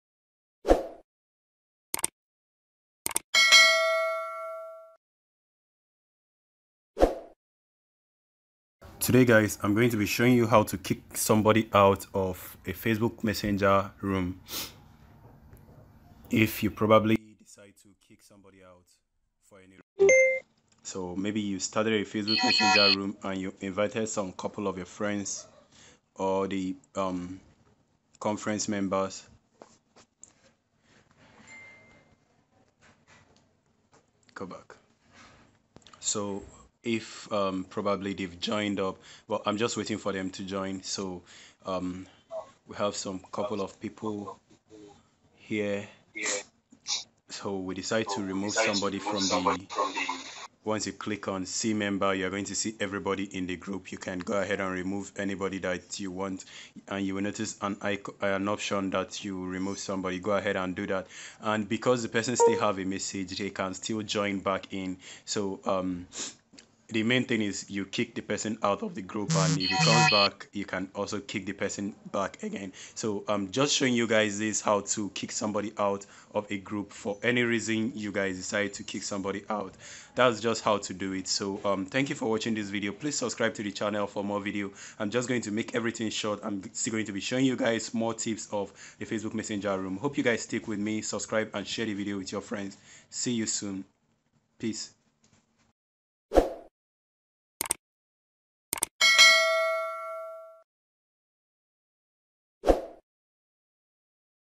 how to kick member from conference meeting on messenger room
As room admin you can remove any member from the video confrence room if you want and can also lock the room from anyone person from entering back after you must ave removed them so watch closely how you can do this, if you already figured out how to do this yourself that is okay you can wait for my next video.

Removing anyone from facebook messenger room is very easy to do with these steps.